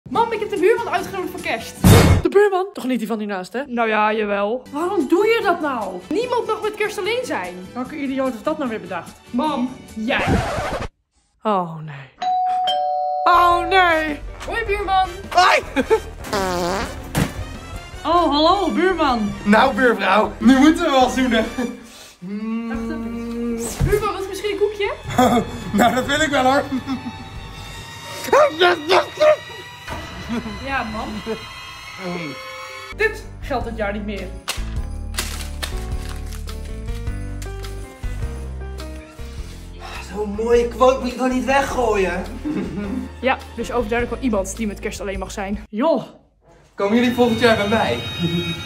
Niemand mag alleen zijn met kerst🎁
Maxime nodigt de buurman uit met kerst, maar daar is de moeder niet zo blij mee...